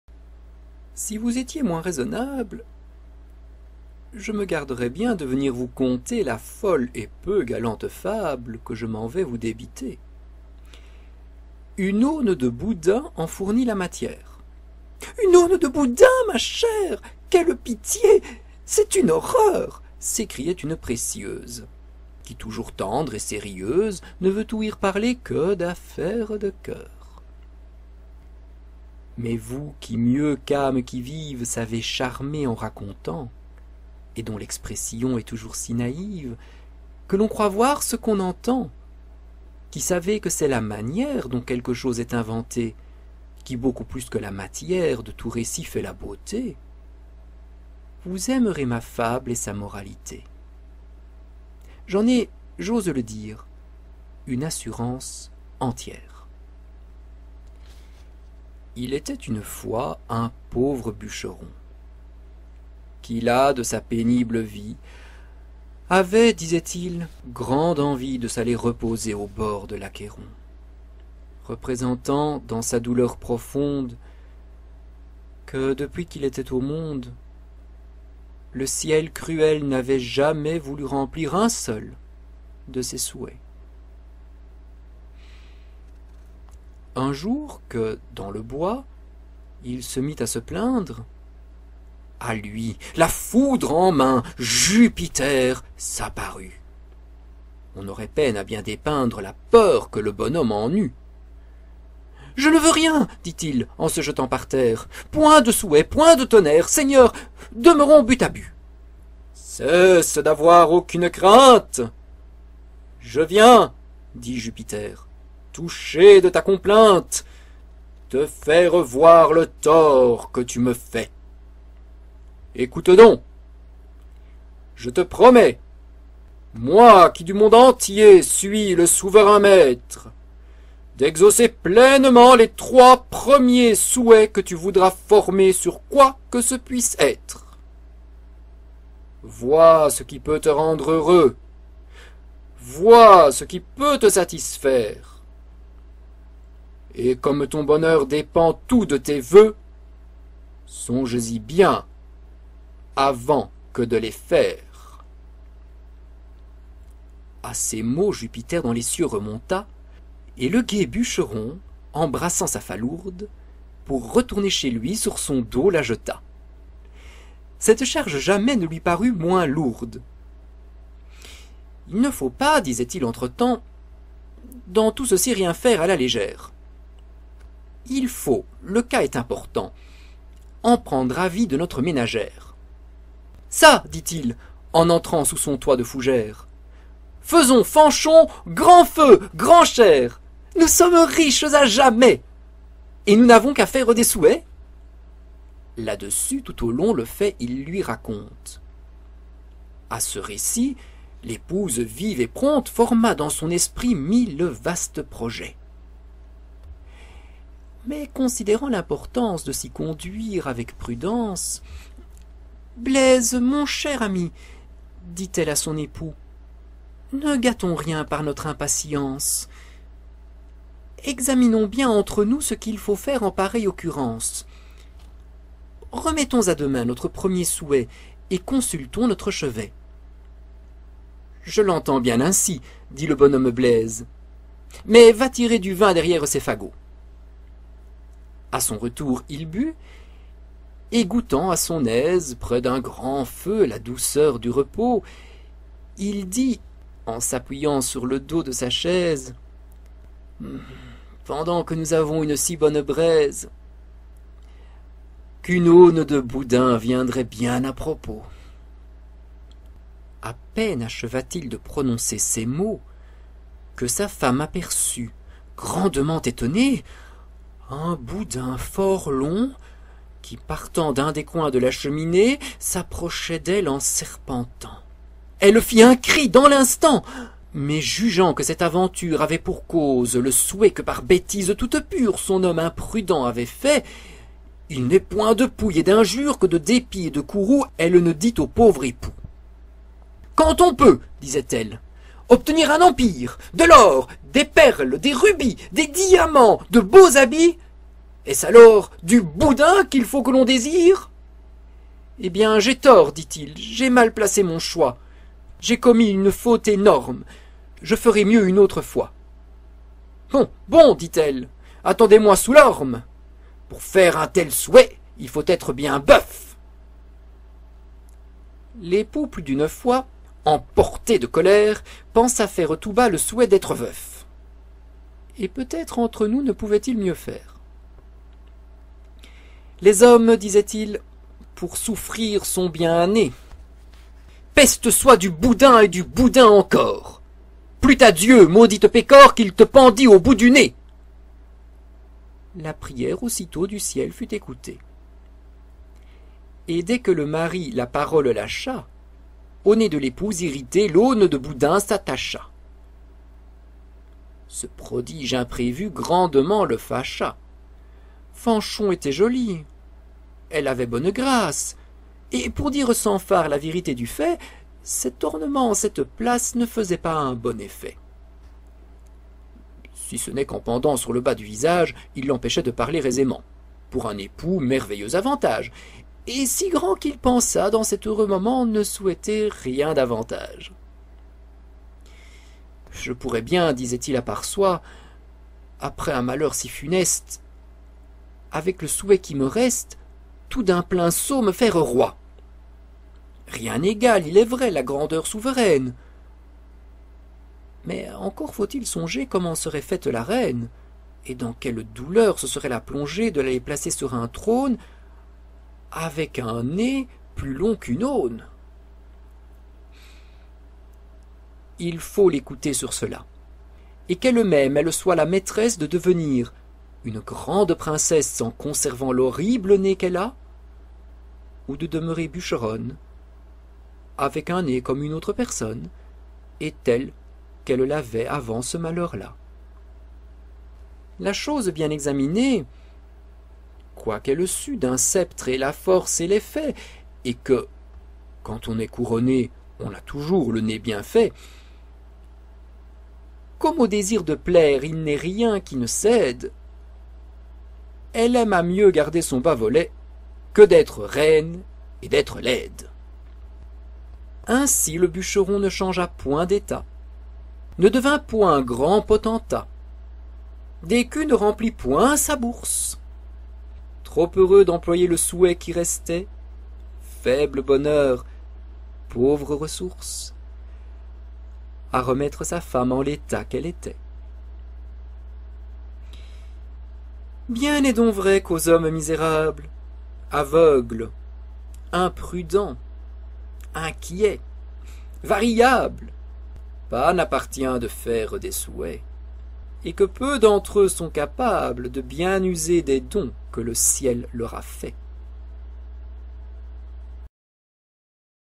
"Les souhaits ridicules" de Charles Perrault
Conte en vers de Charles Perrault (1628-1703).
Lu par Marc Slingeneyer.